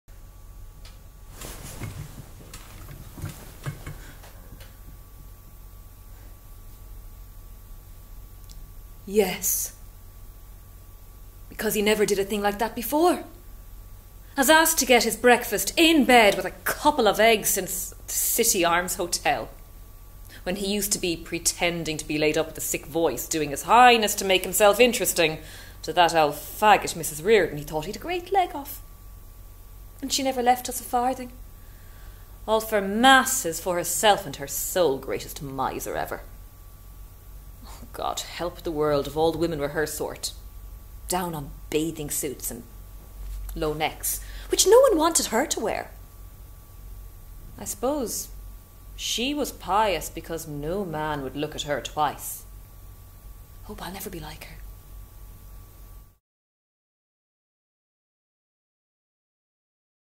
MOLLY trailer Bloomsday Festival Dublin
performed by Tara Breathnach
adapted from James Joyce by Tara Breathnach and Petal Pilley
Coming to Bewley's Cafe Theatre June 2018